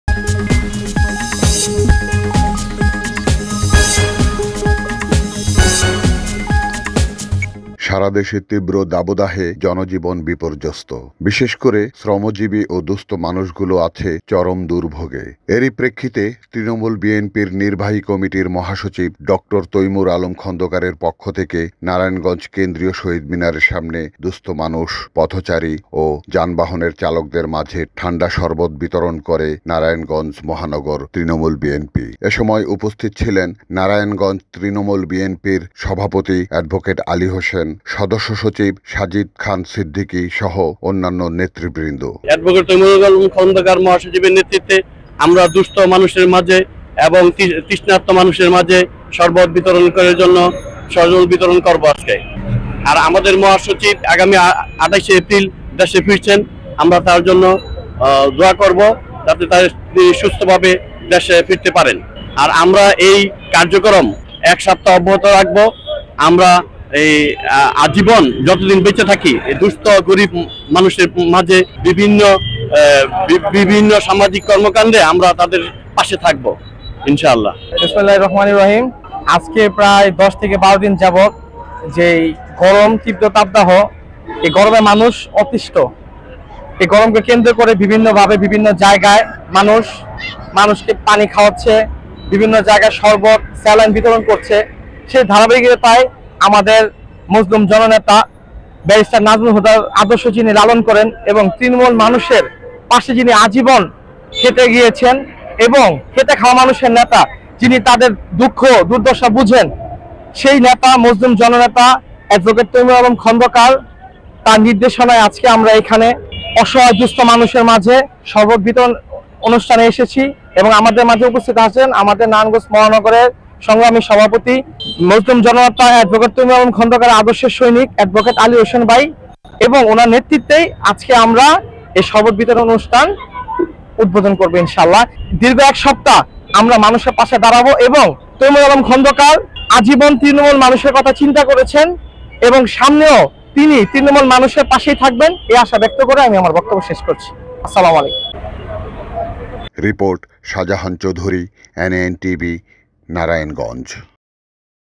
ড. তৈমুর আলমের এর পক্ষ থেকে তীব্র দাবদাহে তৃষ্ণার্তদের মাঝে ঠান্ডা শরবত বিতরণ || NAN TV
Official Address:
76/2 Uttor Chashara, Rajdhani Apurbo Tower,